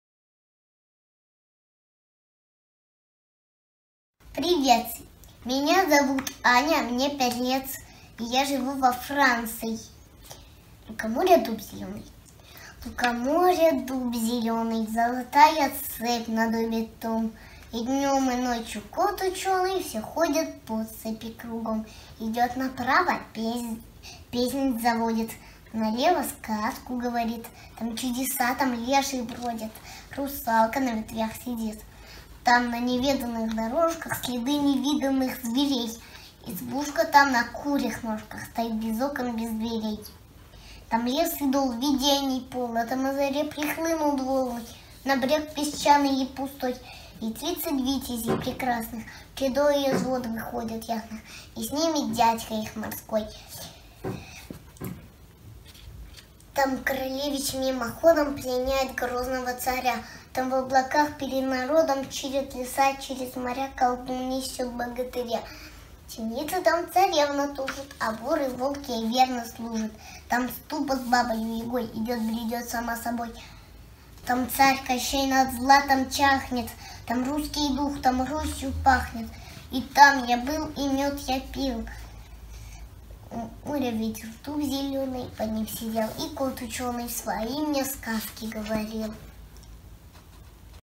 Франция
С ПУШКИНСКИМ ПРИВЕТОМ
из Франции

Пятилетняя Аня Рязанова из Кламара, пригорода Парижа, читает поэтическое вступление к поэме «Руслан и Людмила».